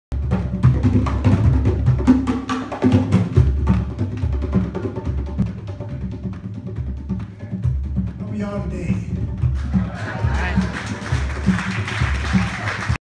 Mysterious Travelers 4: Internal Investigations | Precussion and Polyrhythms
Exotic percussion and polyrhythms inspired by the Music Department's collection of Brazail and Cuba gave the foundational groove for most of the night.